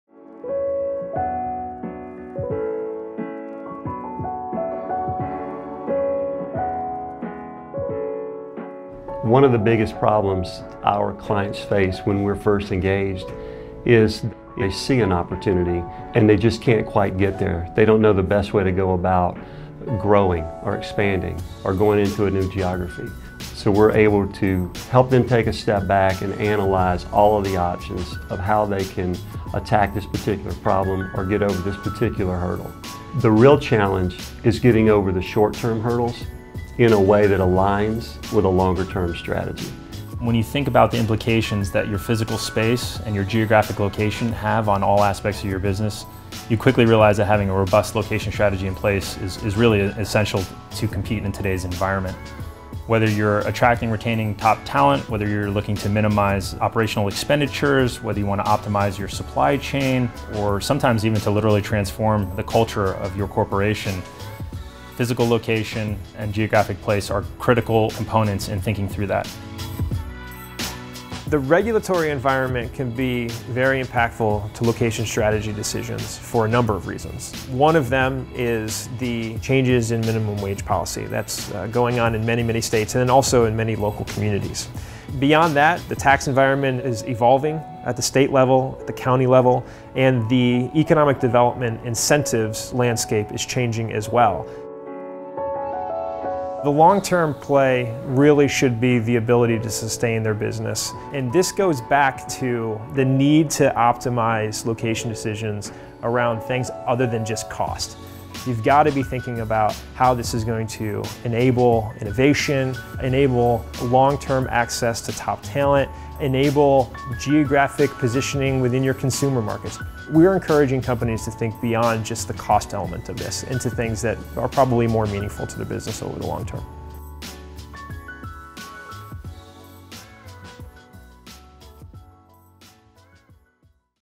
Cresa Viewpoint: Geographic Strategy
Organizations that choose operational locations based primarily on low costs and promising markets for goods and services often overlook critical factors that impact long-term success and failure.